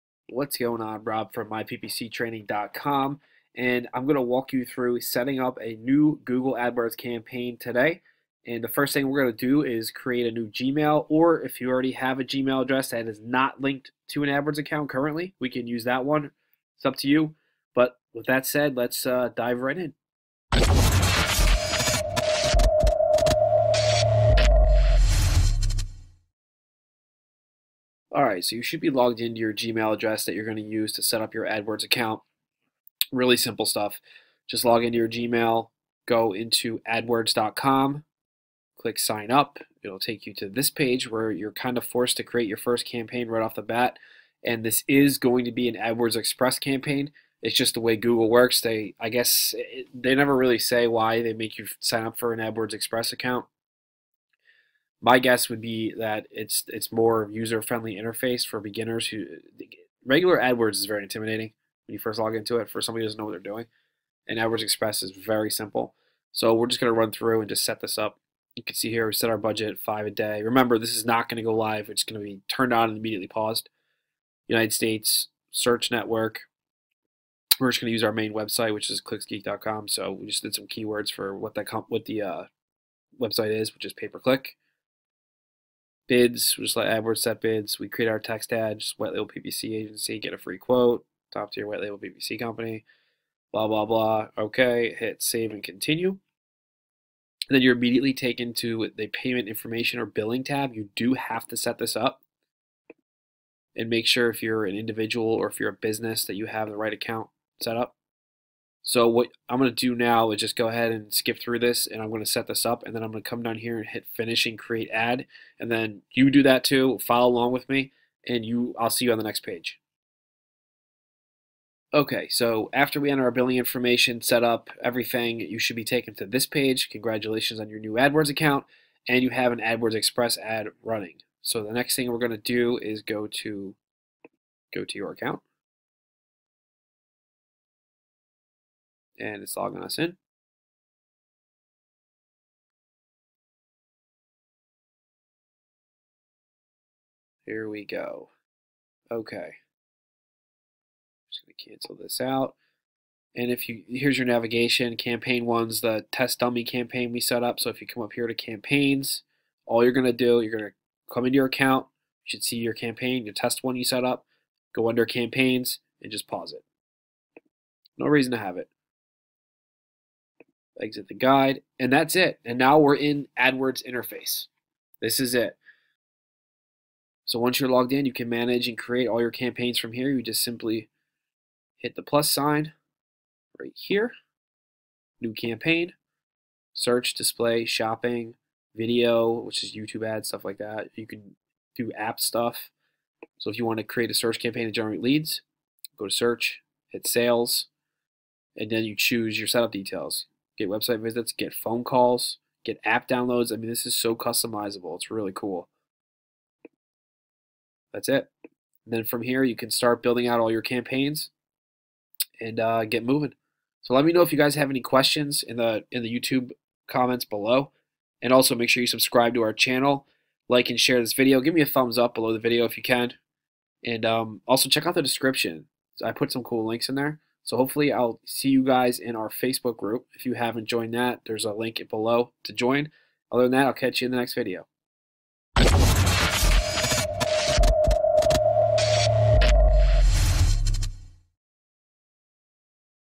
Creating An Adwords Account MUST SEE - New Interface
Creating An AdWords Account 2018 using the new interface. In this video tutorial we show you how to create and setup a new Google AdWords account.

⏱️TIMESTAMPS⏱️
0:00 Intro
0:27 Getting Started with Google AdWords
1:08 Your first campaign
1:38 Payment Information
2:15 Managing your campaign
2:58 Creating new campaigns
3:33 Wrap up